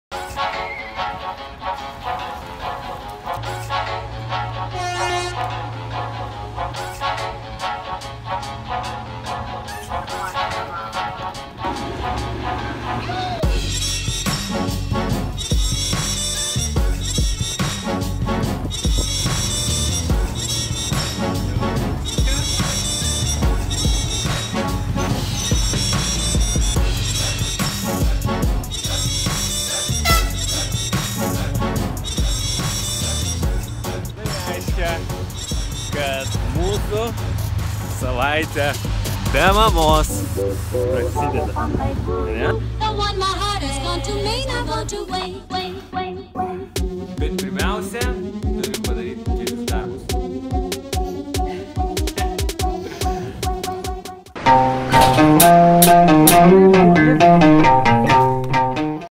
Tetis Patiria: Savaitė be....
Viena labai gera savaitė baigėsi. Aišku, baigėsi ne itin sėkmingai - mūsų šeima vs ROT’o virusas baigė mūšį 0:4. Gavom. Bet prasideda nauja.

Manau, bus ypatinga - Aura išvyksta į Vieną mokytis, mes, tuo tarpu, liekam…